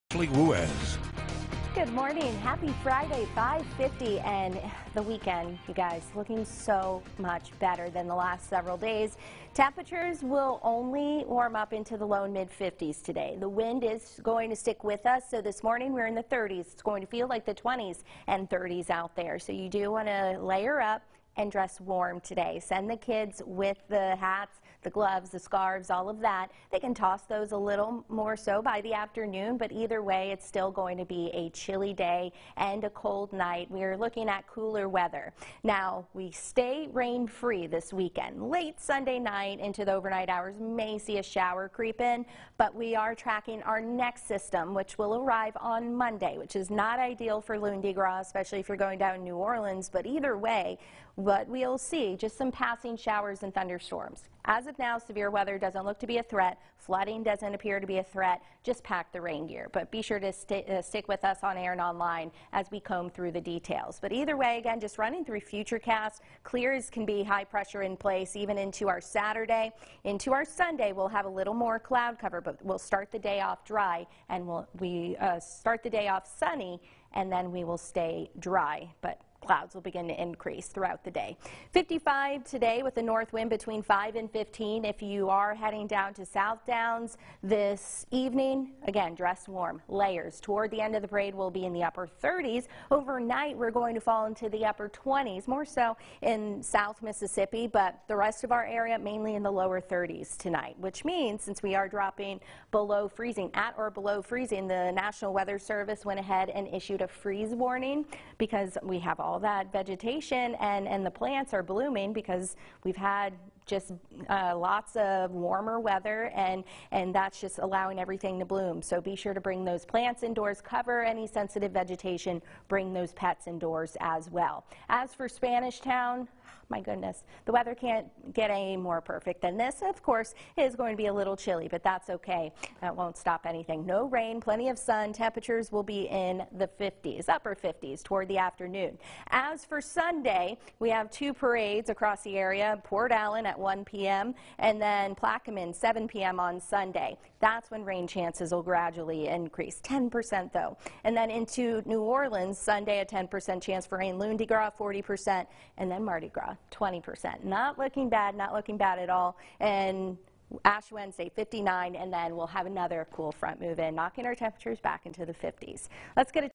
StormTracker Morning Forecast (February 21, 2020)
Meteorologist Ashley Ruiz has your forecast